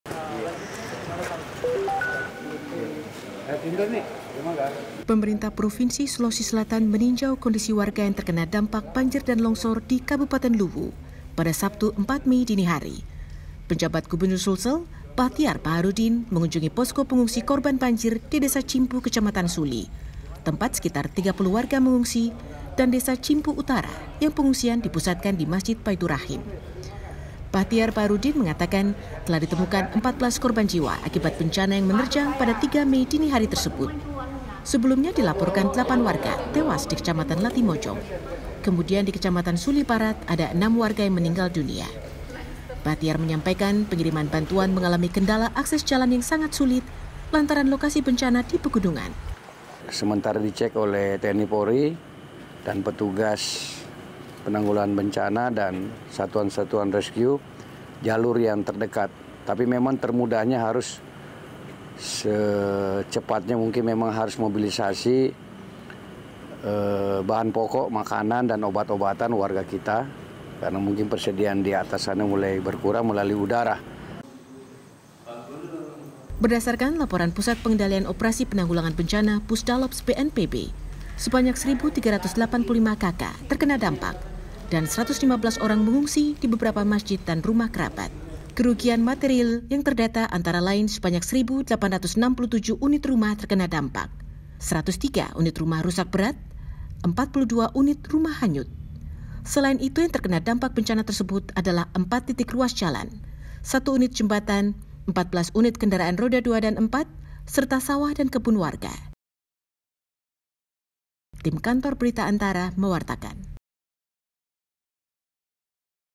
Akses sulit, bantuan korban banjir bandang Sulsel via jalur udara
ANTARA - Pemerintah Provinsi Sulawesi Selatan meninjau kondisi warga yang terkena dampak banjir dan longsor di Kabupaten Luwu, Sulsel, Sabtu (4/5) dini hari. Penjabat Gubernur Sulsel Bahtiar Baharuddin mengatakan telah ditemukan 14 korban jiwa akibat bencana tersebut, dan bantuan makanan serta obat-obatan akan diturunkan melalui jalur udara lantaran akses jalan terputus. (Shintia Aryanti Krisna/Rizky Bagus Dhermawan/Gracia Simanjuntak)